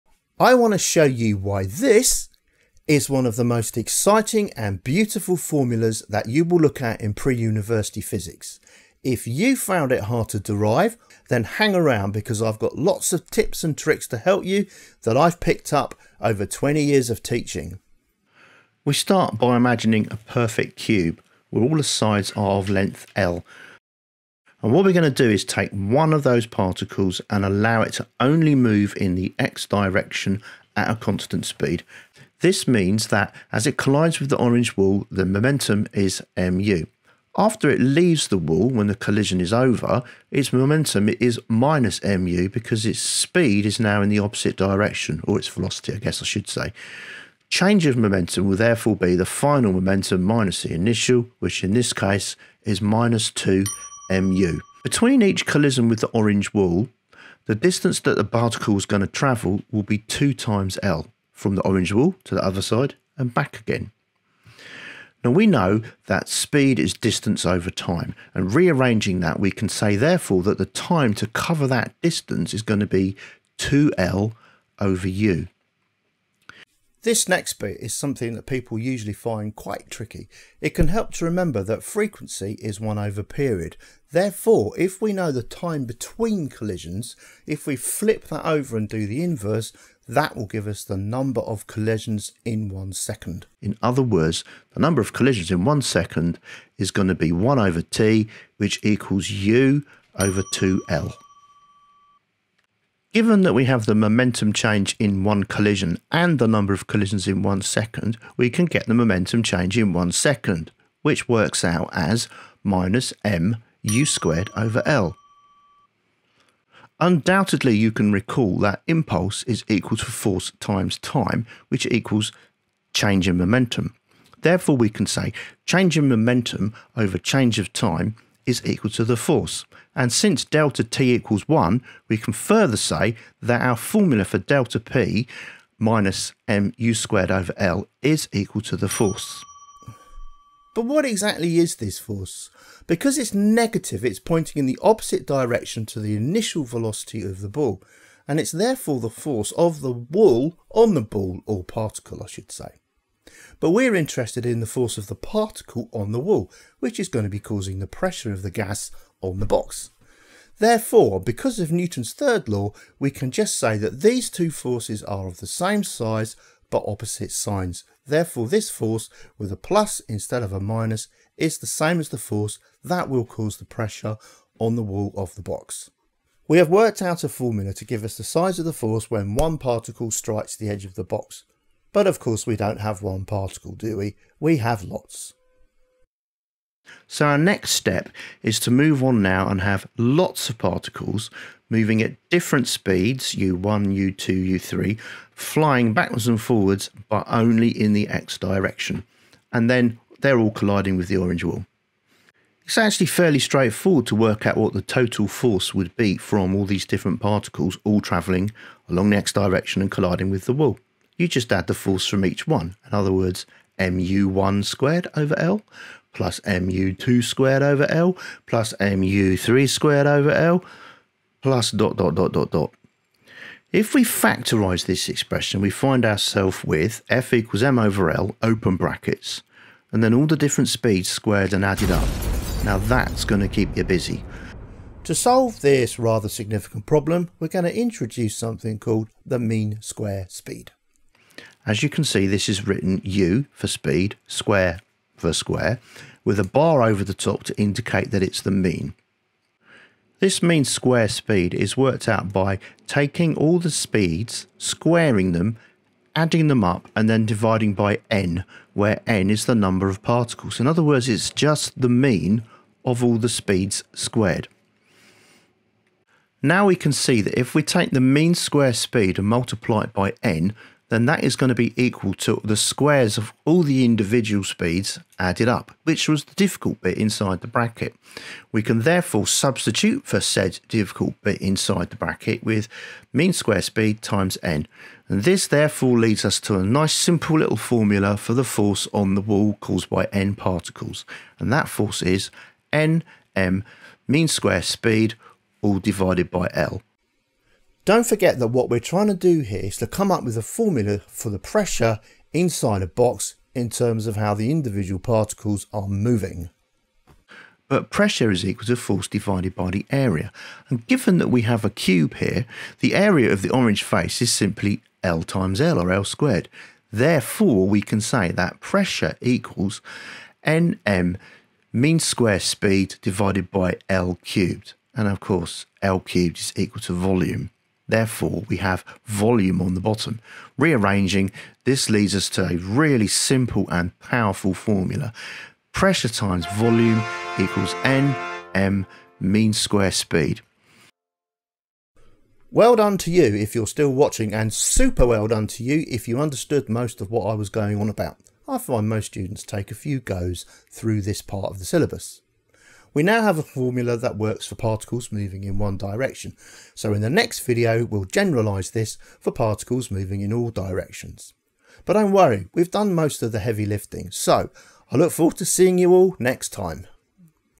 Deriving the Ideal Gas Equation: The Ultimate Guide
Learn how to easily derive the ideal gas equation in this comprehensive physics tutorial. Whether you're studying for A Level Physics, pre-university Physics, or simply interested in understanding thermal physics, this video will guide you through the process step by step. From the pressure of ideal gas to the root mean square and gas laws, this lesson covers it all. Join us for a detailed explanation of gas properties, thermodynamics, and kinetic theory. If you're looking for online tutoring, Enfield Physics Tutor has got you covered. Perfect for physics revision and gaining a solid understanding of ideal gas behavior. Start mastering this fundamental concept today!